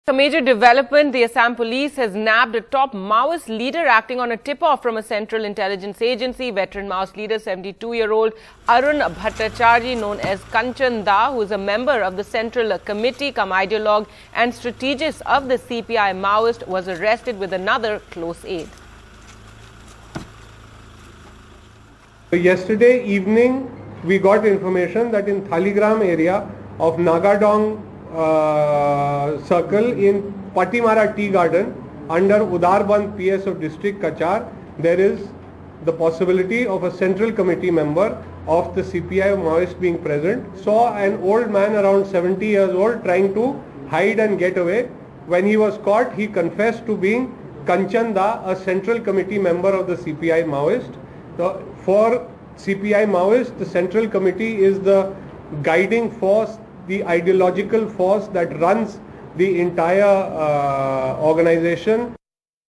CPI (Maoist) Leader 'Kanchan Da' Arrested In Assam: Police
CPI (Maoist) leader and a member of its central committee, Arun Kumar Bhattacharjee, known by his nom de guerre 'Kanchan Da', was arrested in Cachar district of Assam, a top police officer said on Monday. He was arrested from a tea garden under Udharbond police station on Sunday evening along with one of his associates Akash Orang alias Rahul, Guwahati Police Commissioner Harmeet Singh told a press conference here.